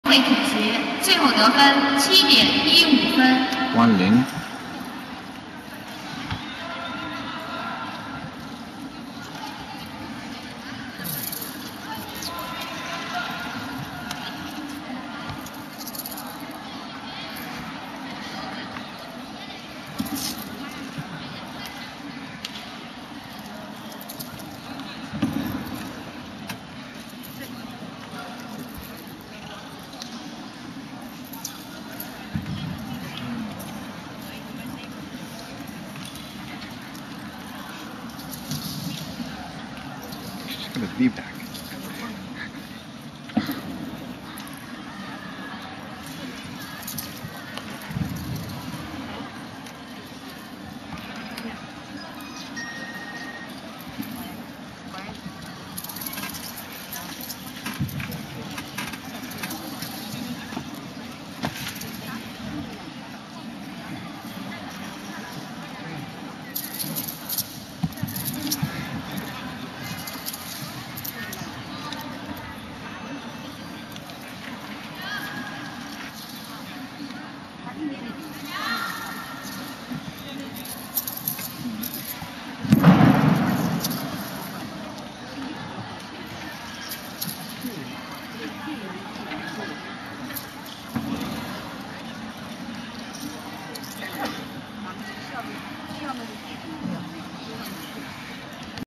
Li Meng Qi - Qiangshu 03 [2006 China Women's Wushu National Qualifiers]
Li Meng Qi competing in Qiangshu (Spear) at the 2006 China Women's Wushu National Qualifiers in Hangzhou, Zhejiang, China on June 8, 2006.  Final Score 7.60